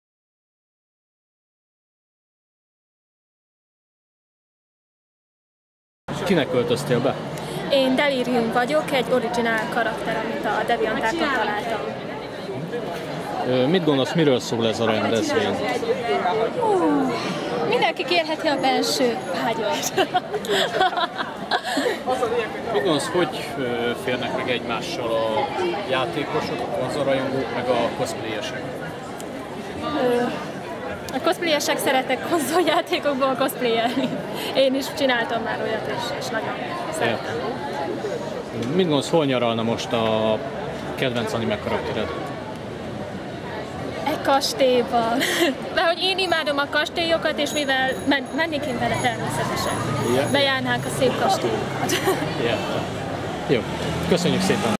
Életképek a 2012-es Cosplayer Expo-ról - 10. rész - ekulturaTV
Kamerás rohambrigádunk a beöltözött rajongókat és az idetévedt látogatókat faggatta.
2012. augusztus 18.